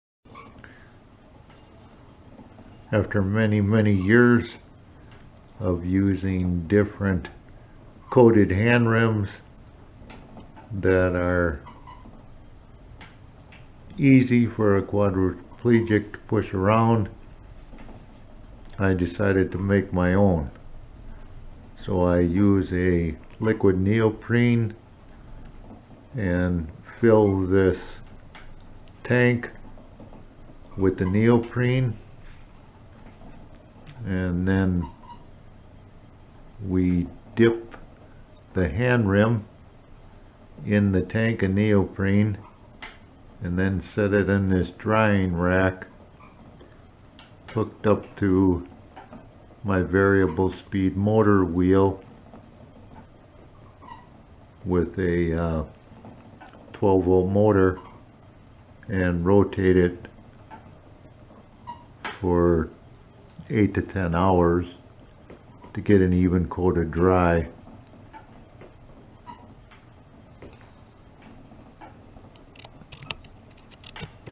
NEOPRENE RUBBER COATED HANDRIM CREATED FOR DISABLED C5-6 SCI QUADRIPLEGIC TO PUSH MANUAL WHEELCHAIR
WITH THE HANDRIM SECURED TO THE WOODEN SPIDER IT IS DIPPED IN A TROUGH FILLED WITH LIQUID RUBBER AND ROTATED BY HAND TO COAT THE ENTIRE HANDRIM AS THICK AND EVENLY AS POSSIBLE. THE SPIDER WITH HANDRIM ATTACHED IS THEN PLACED ON A DRYING TROUGH AND ROTATED SLOWLY FOR 8-10 HOURS. THEN A 2ND AND 3RD COAT IS APPLIED.